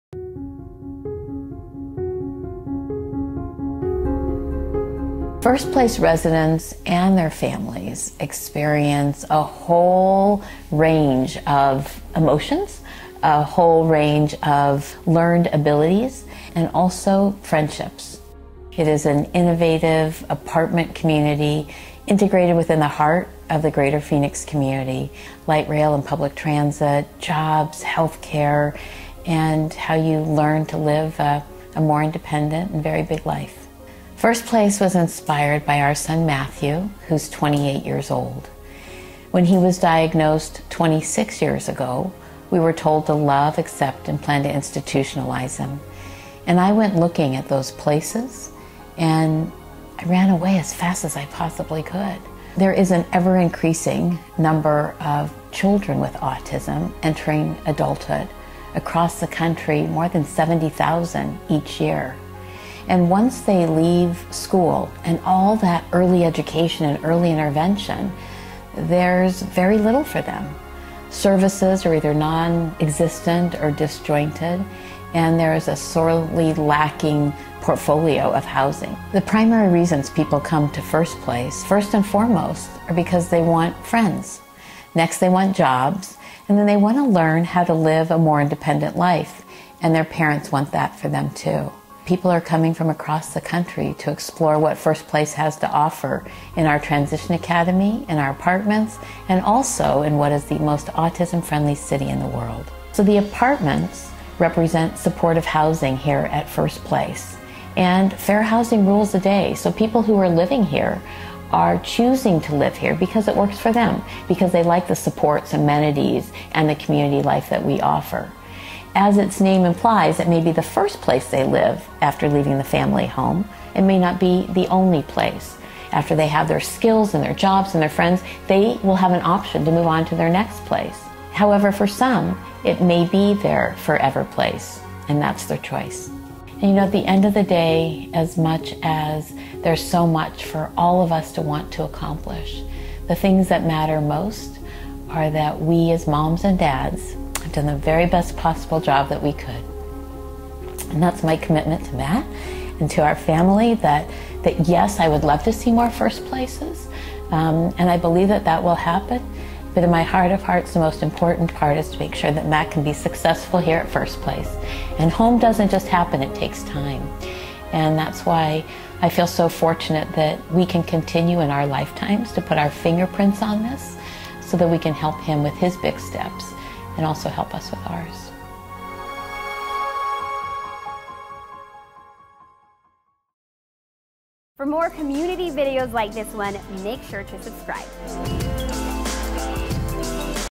First Place
First Place–Phoenix, a housing property designed to help adults with autism and other different abilities, with a mission of building independent lives within a supportive, inclusive community where they can make friends, learn life skills, pursue employment and endless possibilities. DM23